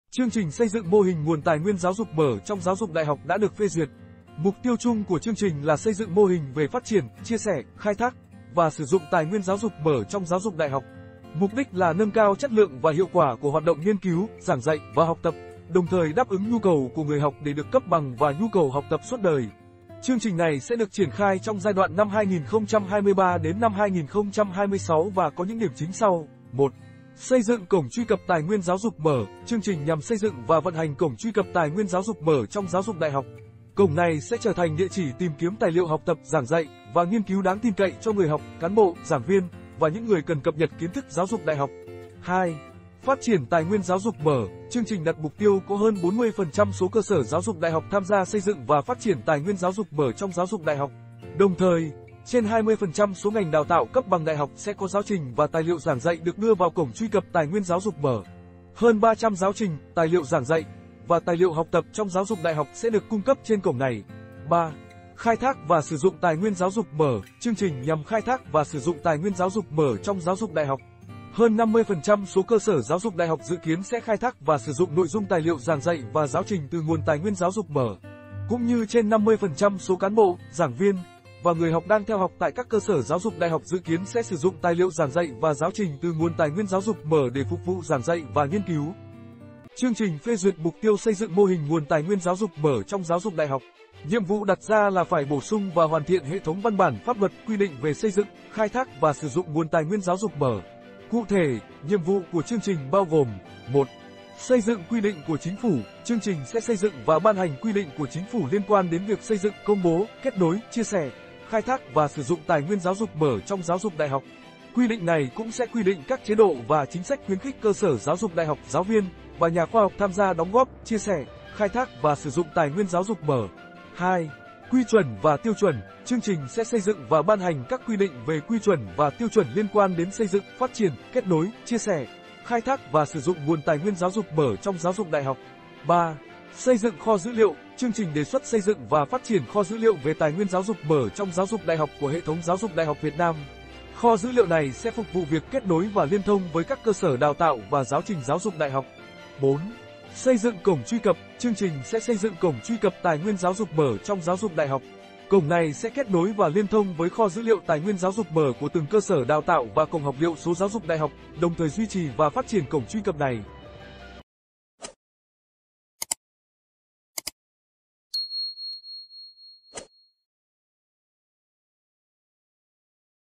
PHÊ DUYỆT CHƯƠNG TRÌNH XÂY DỰNG MÔ HÌNH NGUỒN TÀI NGUYÊN GIÁO DỤC MỞ
Chương trình xây dựng mô hình nguồn tài nguyên giáo dục mở trong giáo dục đại học đã được phê duyệt. Mục tiêu chung của chương trình là xây dựng mô hình về phát triển, chia sẻ, khai thác, và sử dụng tài nguyên giáo dục mở trong giáo dục đại học. Mục đích là nâng cao chất lượng và hiệu quả của hoạt động nghiên cứu, giảng dạy, và học tập, đồng thời đáp ứng nhu cầu của người học để được cấp bằng và nhu cầu học tập suốt đời. Chương trình này sẽ được triển khai trong giai đoạn 2023 – 2026 và có những điểm chính sau: 1. Xây Dựng Cổng Truy Cập Tài Nguyên Giáo Dục Mở: Chương trình nhằm xây dựng và vận hành cổng truy cập tài nguyên giáo dục mở trong giáo dục đại học. Cổng này sẽ trở thành địa chỉ tìm kiếm tài liệu học tập, giảng dạy, và nghiên cứu đáng tin cậy cho người học, cán bộ, giảng viên, và những người cần cập nhật kiến thức giáo dục đại học. 2. Phát Triển Tài Nguyên Giáo Dục Mở: Chương trình đặt mục tiêu có hơn 40% số cơ sở giáo dục đại học tham gia xây dựng và phát triển tài nguyên giáo dục mở trong giáo dục đại học. Đồng thời, trên 20% số ngành đào tạo cấp bằng đại học sẽ có giáo trình và tài liệu giảng dạy được đưa vào cổng truy cập tài nguyên giáo dục mở. Hơn 300 giáo trình, tài liệu giảng dạy, và tài liệu học tập trong giáo dục đại học sẽ được cung cấp trên cổng này. 3. Khai Thác Và Sử Dụng Tài Nguyên Giáo Dục Mở: Chương trình nhằm khai thác và sử dụng tài nguyên giáo dục mở trong giáo dục đại học. Hơn 50% số cơ sở giáo dục đại học dự kiến sẽ khai thác và sử dụng nội dung tài liệu giảng dạy và giáo trình từ nguồn tài nguyên giáo dục mở. Cũng như trên 50% số cán bộ, giảng viên, và người học đang theo học tại các cơ sở giáo dục đại học dự kiến sẽ sử dụng tài liệu giảng dạy và giáo trình từ nguồn tài nguyên giáo dục mở để phục vụ giảng dạy và nghiên cứu.